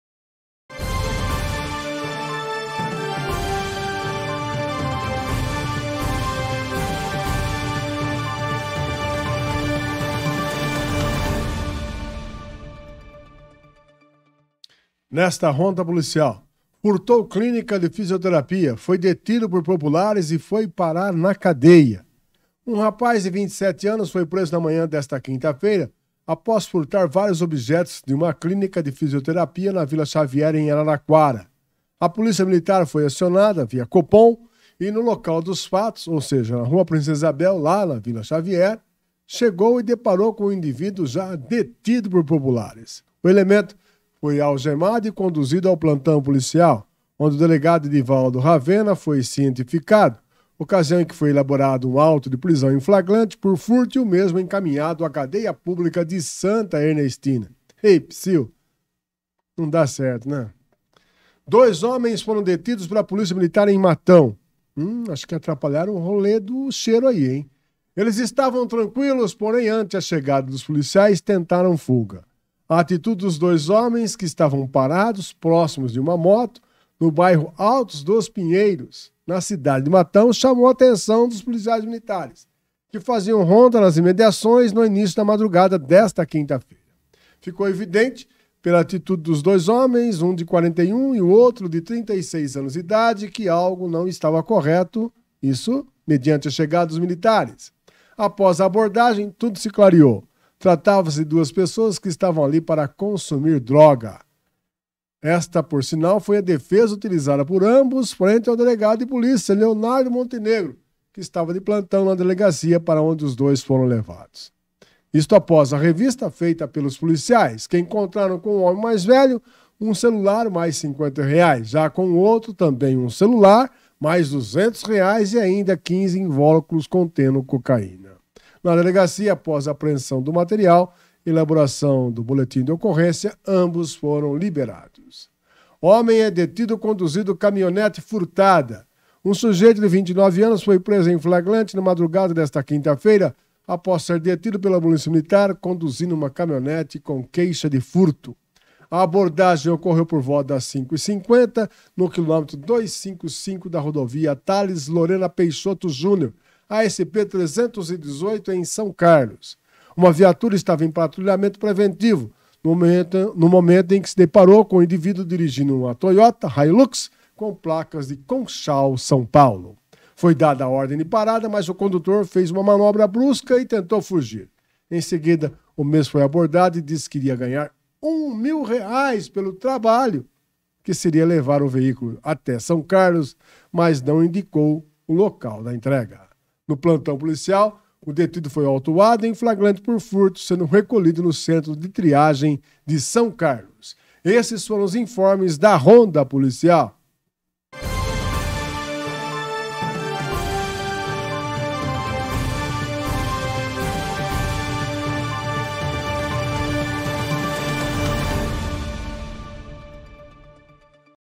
Ronda Policial
Ladrão foi detido por populares na Vila Xavier.